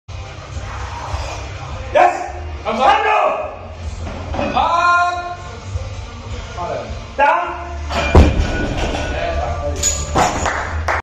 225 kg deadlift comp opener
i was really sore so it didn't move as fast as i wanted it to